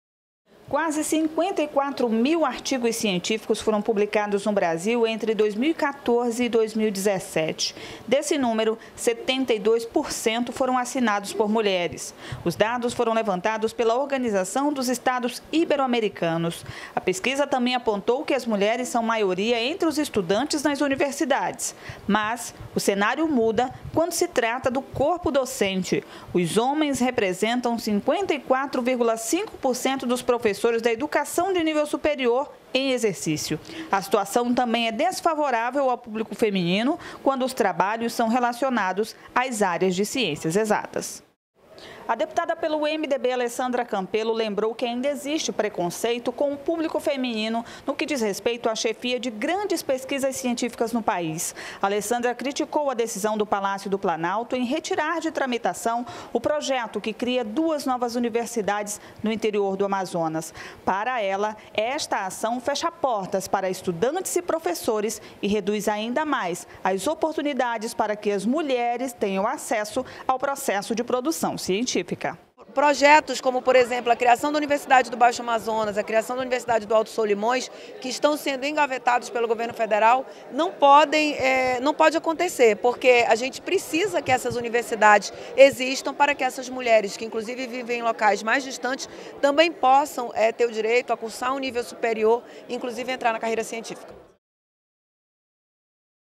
72% dos artigos científicos são assinados por mulheres no Brasil
72% dos artigos científicos publicados no Brasil entre 2014 e 2017 foram assinados por mulheres. Para a deputada pelo MDB, Alessandra Campelo, os dados apontam a participação feminina de forma mais efetiva nos processos de pesquisas cientificas em todo o país. Mas Alessandra alertou que ainda é necessário um maior empenho para que as mulheres possam de fato chefiar grandes pesquisas no Brasil.